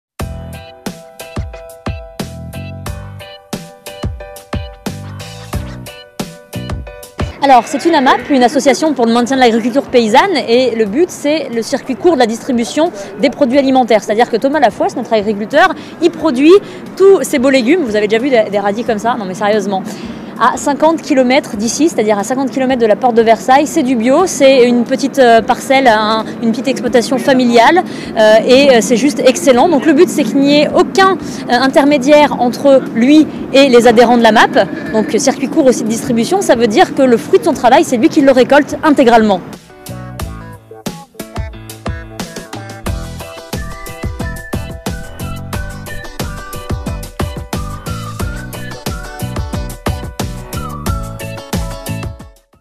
amapà BFMTV; RMC, RMC SPORT, BFM BUSINESS, NEXTRABIOTV
Les journalistes modifient leurs pratiques et font preuve d'exemplarité.
Vives les AMAP!

avec Fanny Agostini, Thomas Leonetti et Raphael Maillochon